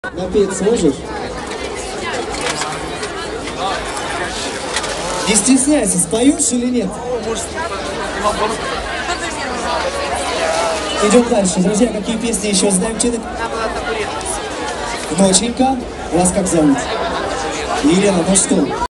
я на дні міста Дніпро
Поки говорить Ведучий на концерті